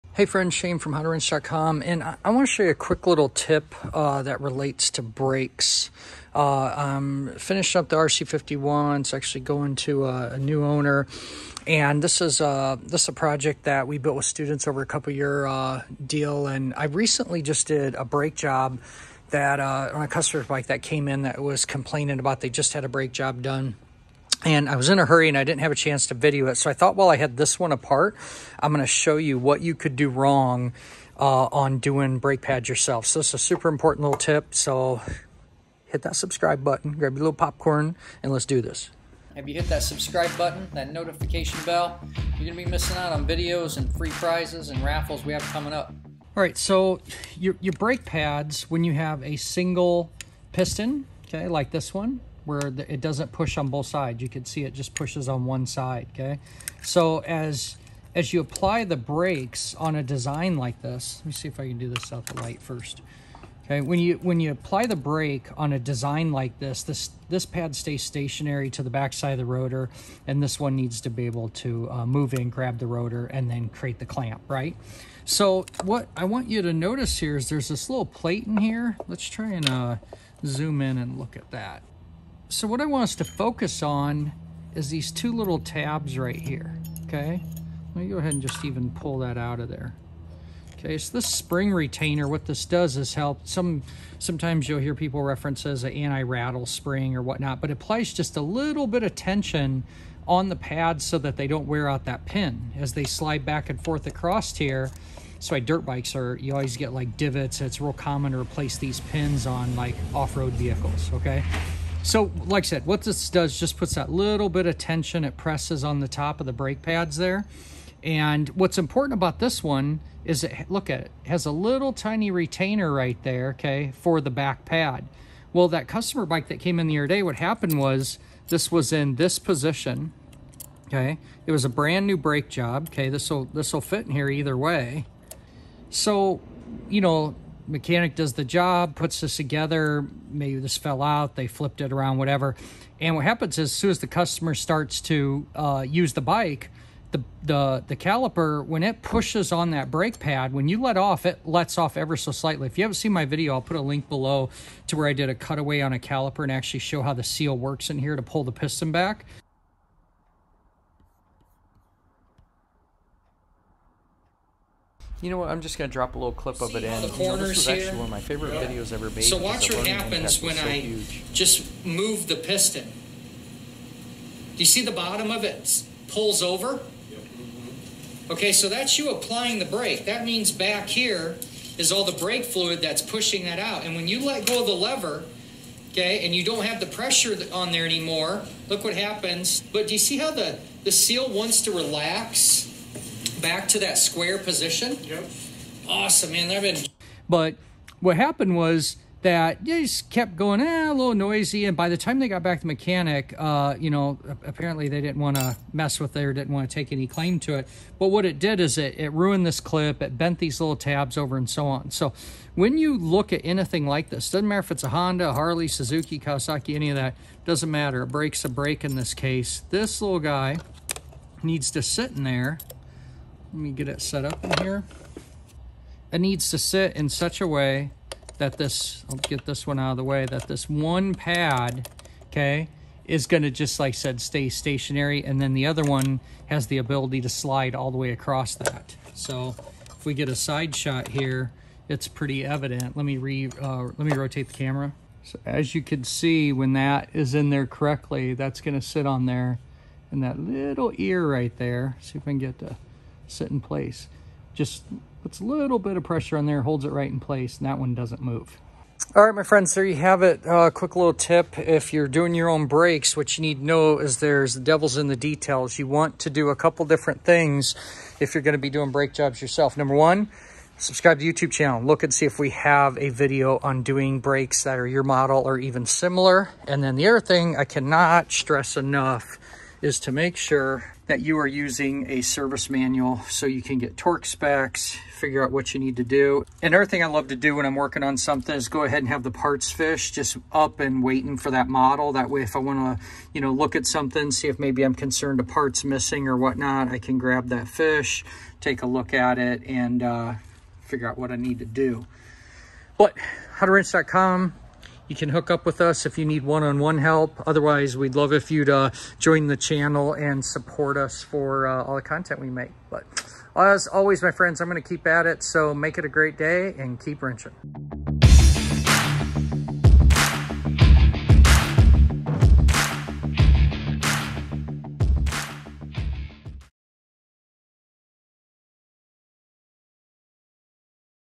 Brake pad installation gone wrong. DIY brake pad Pro Tip. What's that spring for anyway?
Use these tools links to support How2Wrench and know we use the same tools as seen in the videos!
Keep Wrenching,
Shane Conley
Instructor/Motivator/Motorcyclist